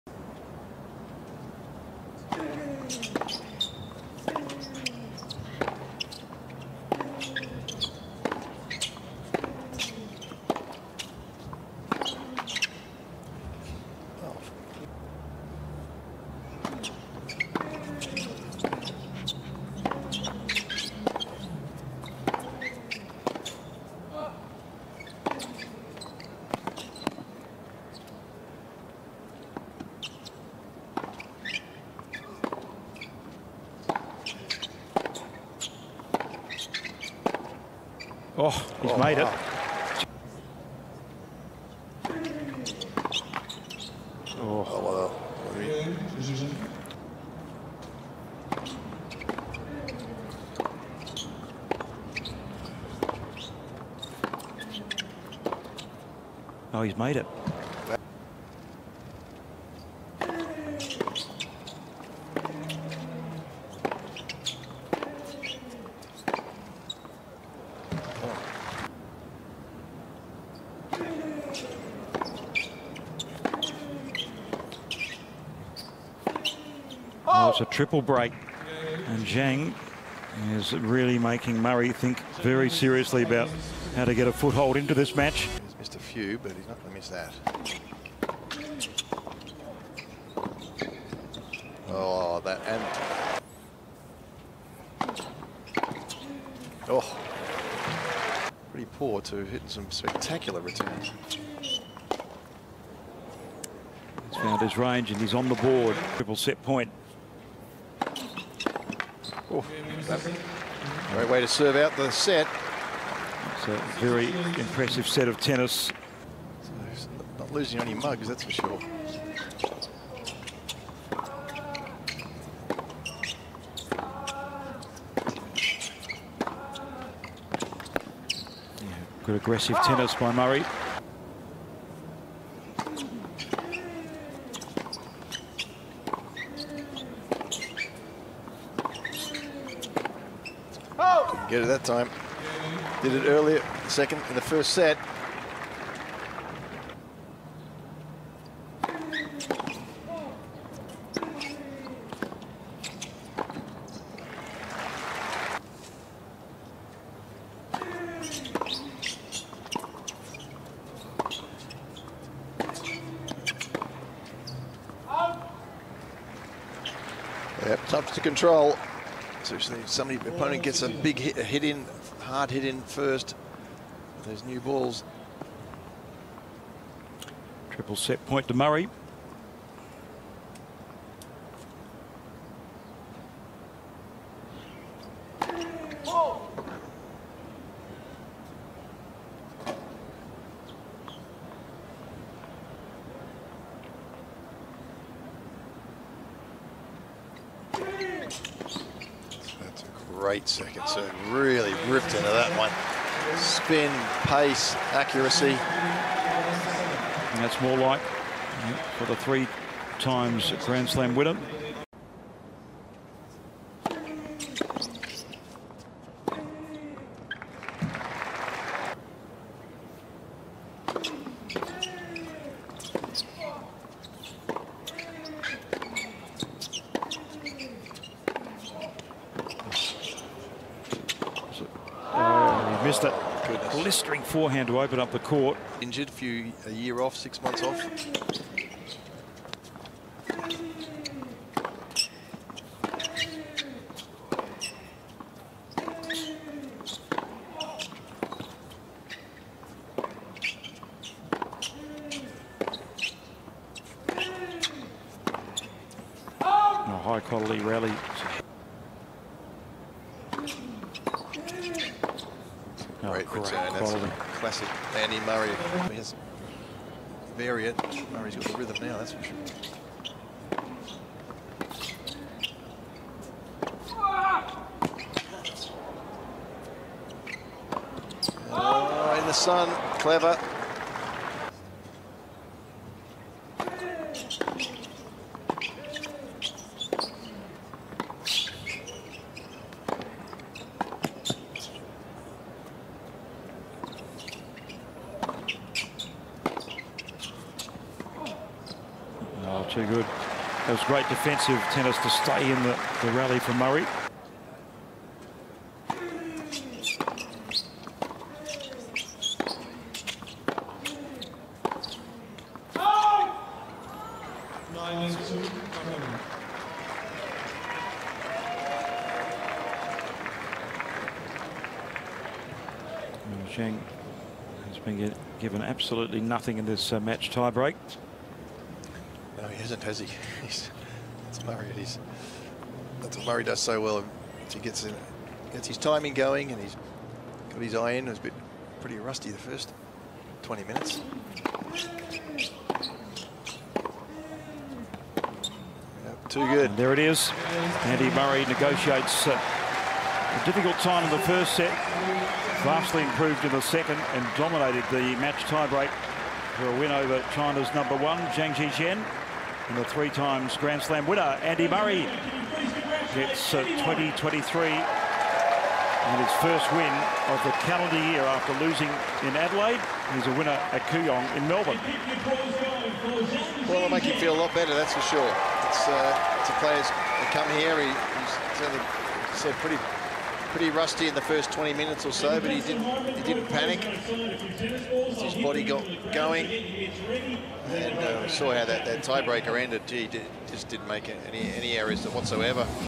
Andy Murray vs Zhizhen Zhang | Kooyong Classic 2023
Highlights of the exhibition match between Andy Murray and Zhizhen Zhang  in the Kooyong Classic in Kooyong Lawn Tennis Club, Australia